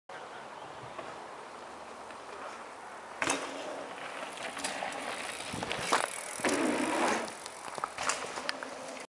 Duthie Hill HLC Line
The start of the HLC line at Duthie Hill mountain bike park, with the drop in and banana hammock.